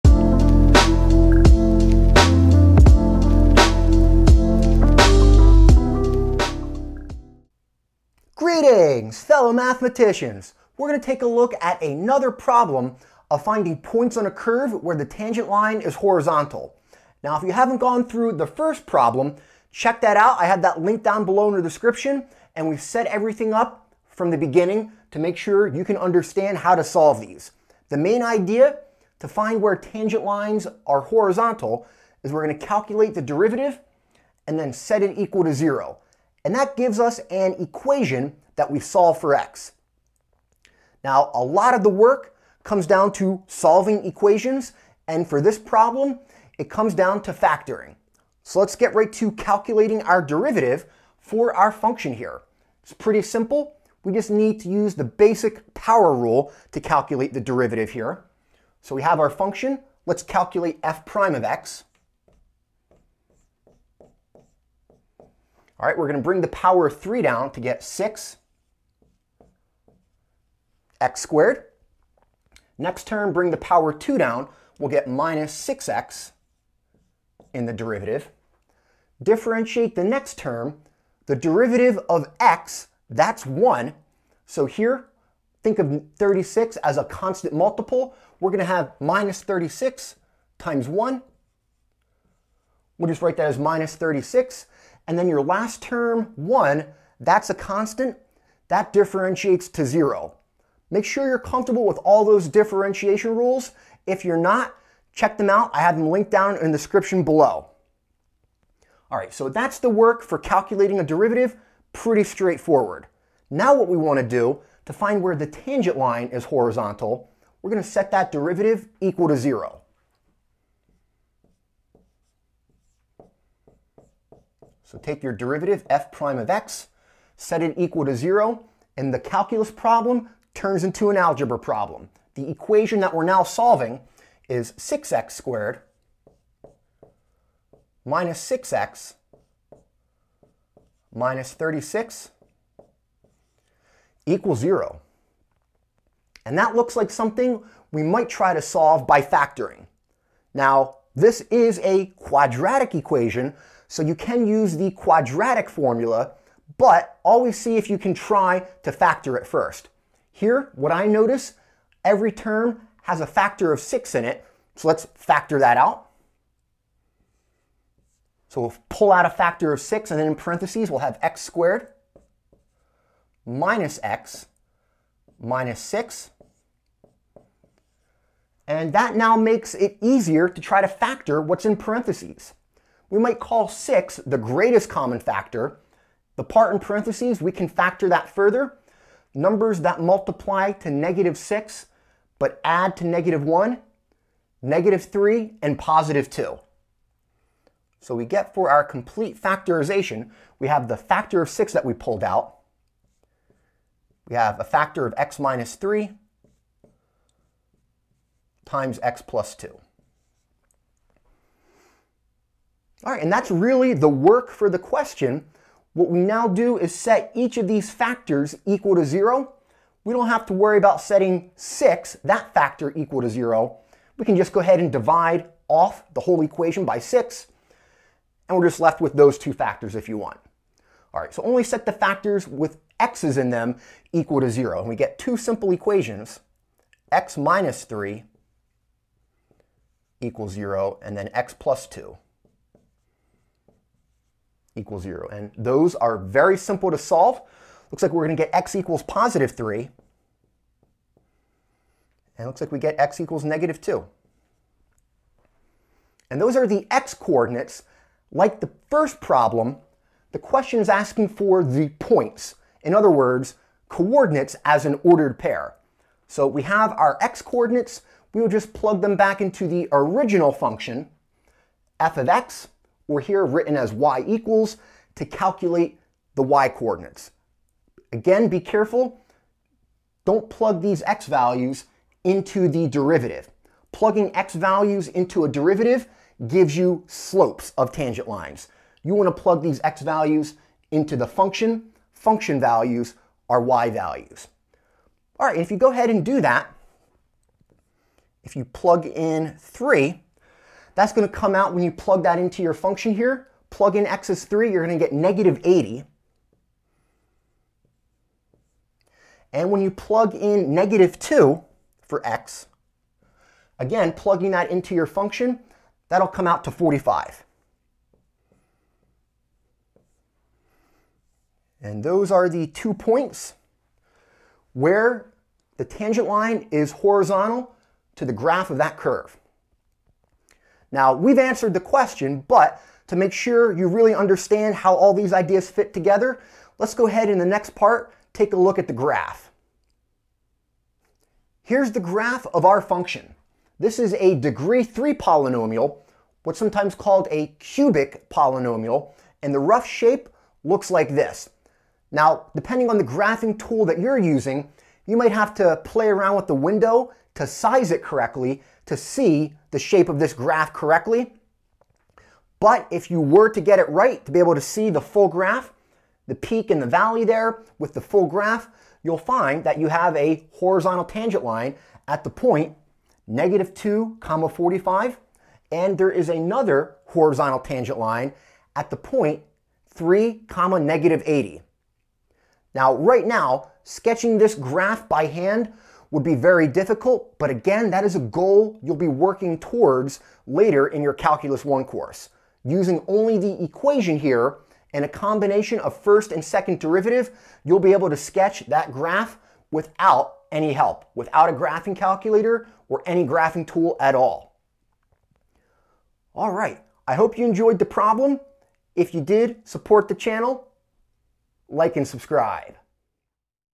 Horizontal Tangent Line with Basic Differentiation Rules Problem 2 (Calculus 1)
Finding the points on the curve where the tangent line is horizontal comes down to solving an equation, by setting the derivative equal to 0. For this problem that leads to a simple quadratic equation that we can solve by factoring. This is good problem to combine using basic differentiation rules together with some fundamental ideas from algebra! At the end, we provide a sketch of the graph so that you can really understand what you're calculating. More problems are linked below.